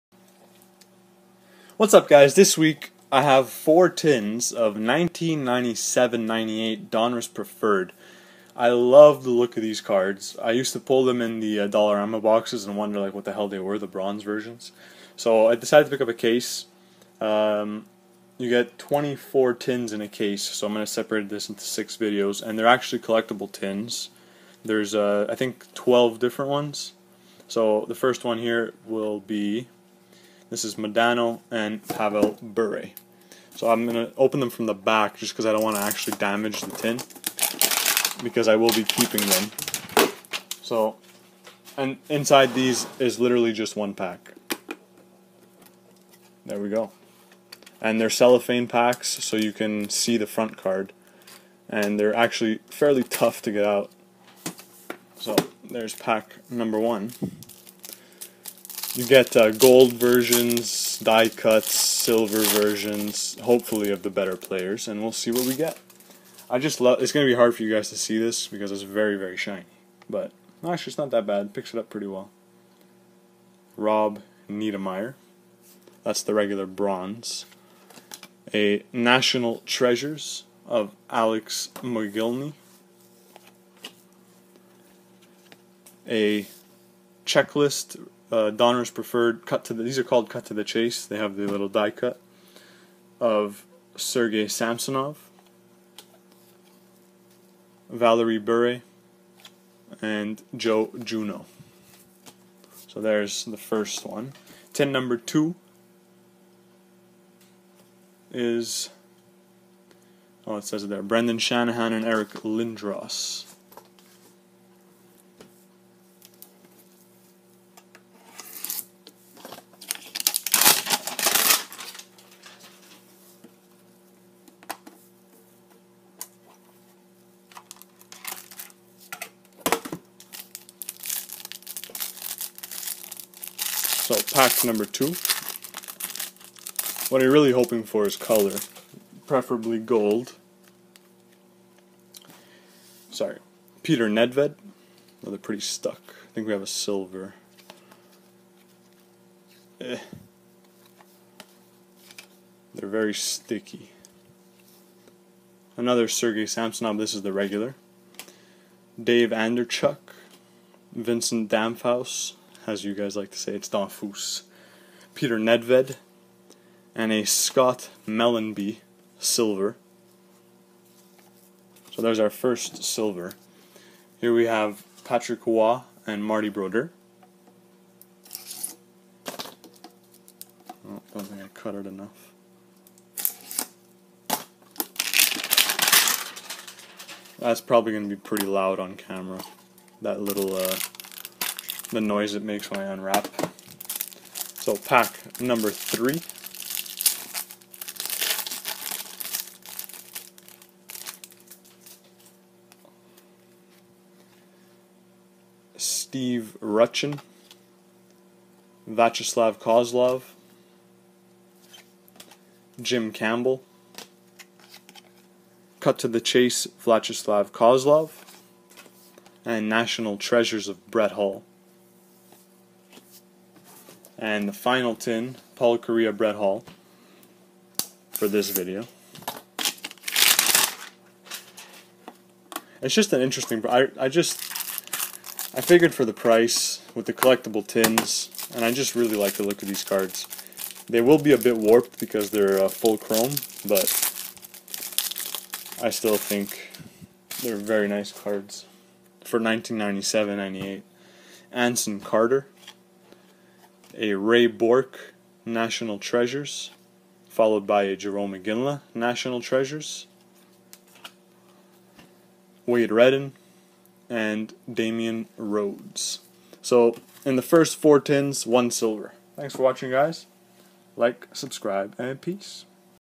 4 Tin Packs of 1997-98 Donruss Preferred Hockey
These tins came out to about $2 each so I figured I'd pick some up. Nice to get a silver out of the first 4 but hopefully there will be some golds to come.